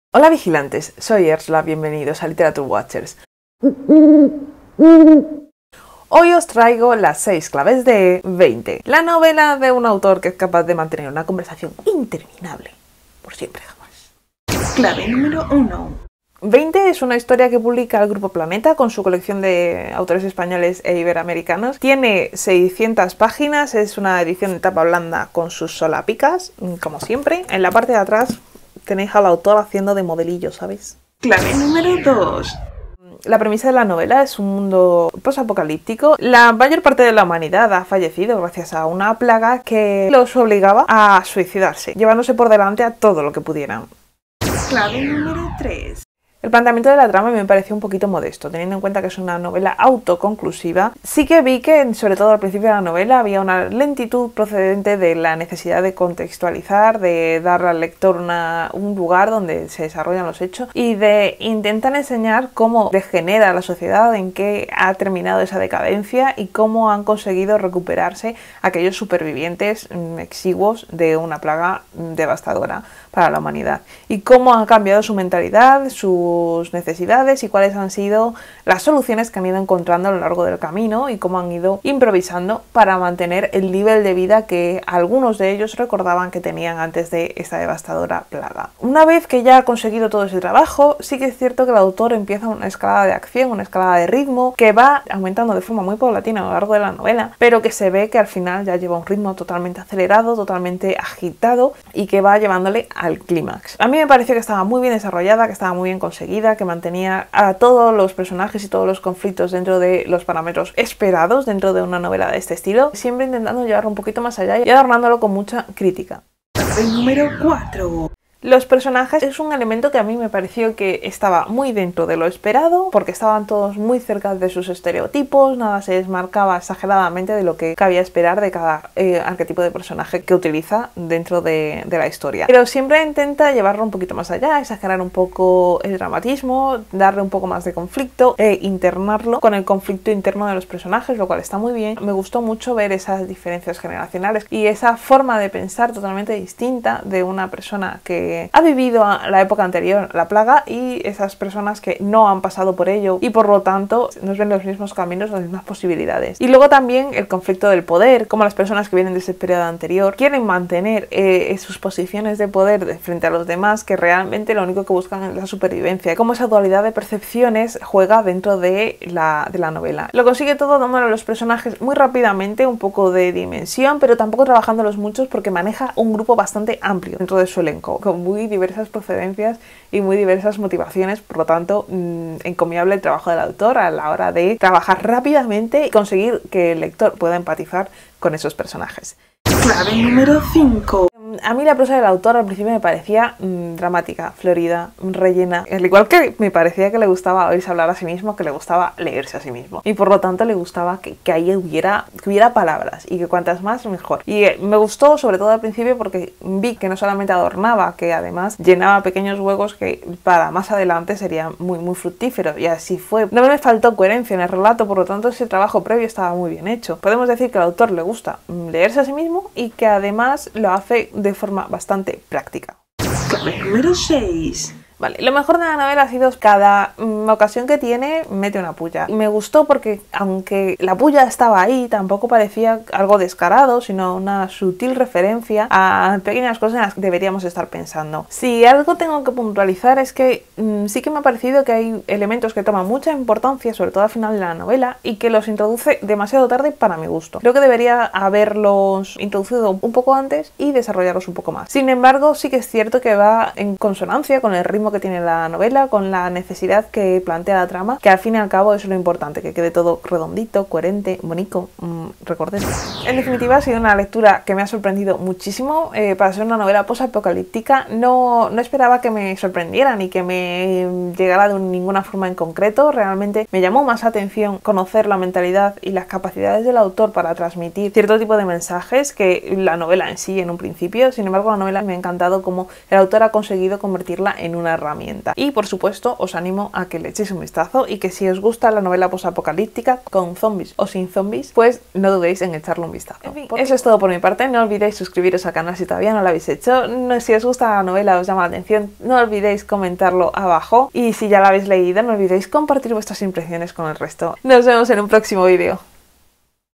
6 Claves de... | Veinte de Manel Loureiro
Hola vigilante,
¡Bienvenido a un nuevo libro! En esta ocasión es un poco más largo que los demás vídeos de esta serie pero esta novela se lo merece. Es decir, que me he líado a hablar y ha quedado un poco más largo...
Espero que os guste y si os animáis a leer la novela me lo contáis en los comentarios.